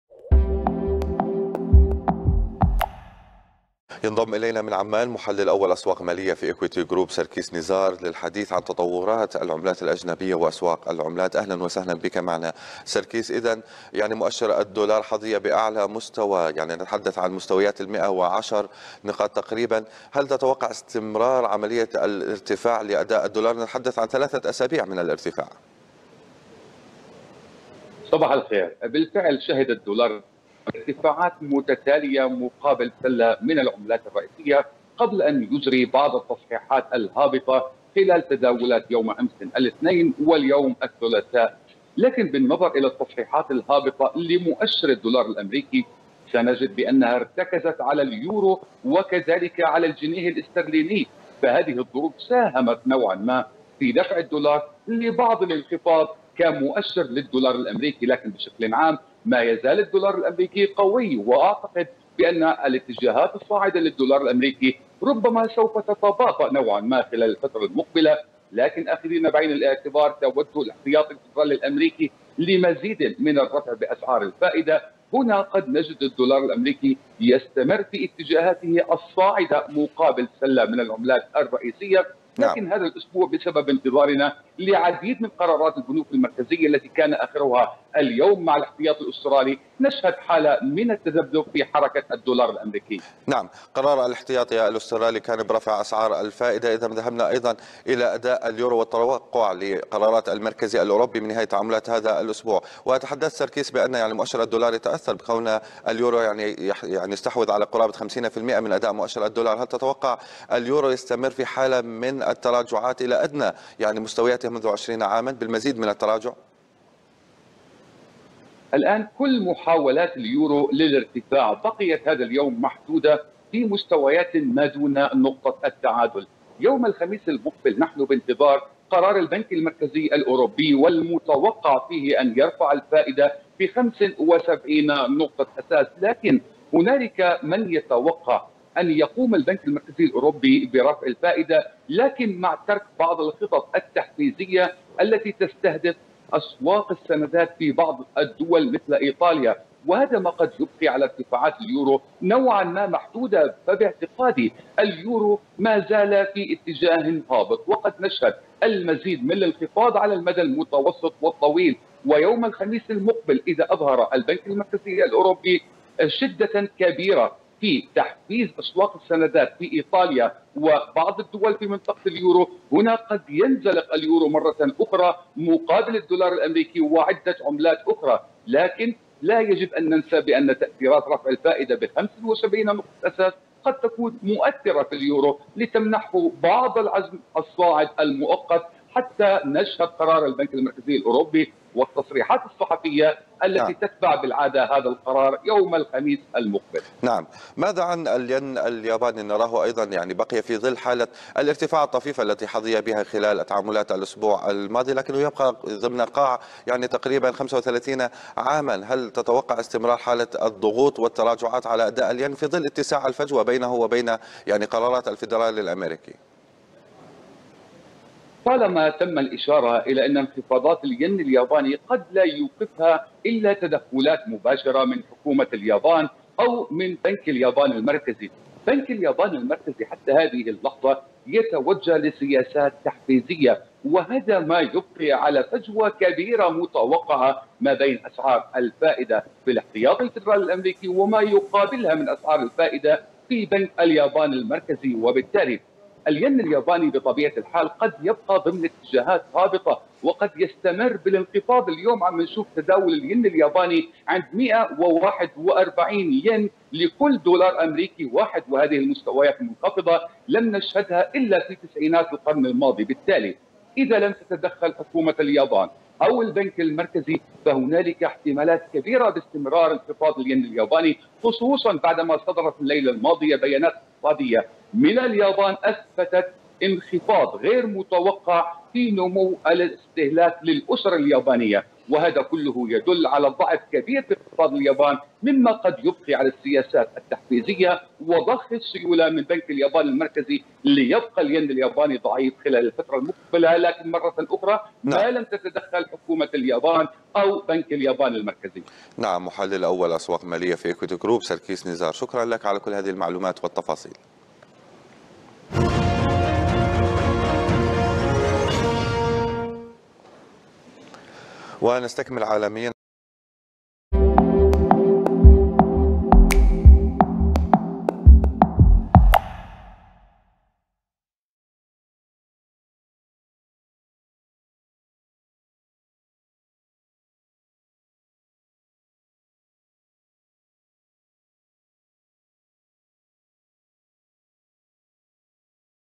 الين الياباني مستمر بانهياره، والمستثمرون ينتظرون قرار المركزي الأوروبي لتحديد اتجاه اليورو
سلسلة من قرارات الفائدة هذا الأسبوع تجعل من تحرّكات الدولار متباينة أمام سلّة العملات
المركزي الأوروبي قد يرفع الفائدة 75 نقطة أساس، بعدما رفع الاحتياطي الأسترالي الفائدة 50 نقطة. اما الفيدرالي الأمريكي، فمن المرجّح أن يرفع الفائدة 75 نقطة أساس.
الين الياباني قد لا يستطيع التوقّف عن الانخفاض ما لم تتدخّل حكومة اليابان أو المركزي الياباني، فيما ارتفاعات اليورو قد تبقى محدودة نوعا ًما